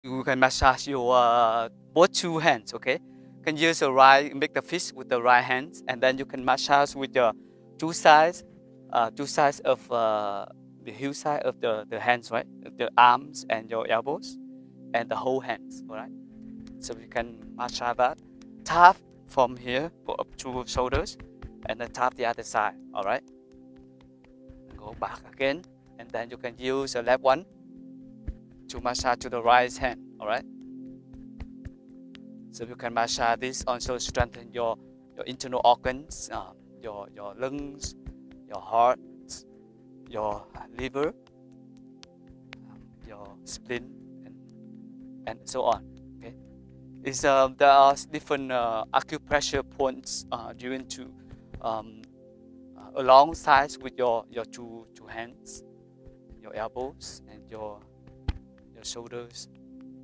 Simple Way to MASSAGE HANDS | Qigong Massage to Keep Hands Strong and Relaxed
This is the 5 Minute Qigong Massage Hands al
Practicing this Qigong will help you to Relax Hands, Palms, Release Tensions of Hands.
You can Practice 5 minutes during Break Time, and before going to bed. It is wonderful to take a break from internet, computer, and devices to enjoy this exercise

Video recorded, edited, and Performed by  Thich Man Tue ( Br Insight)

Enjoy Your Daily Qigong Meditation with Joy and Peace at Home.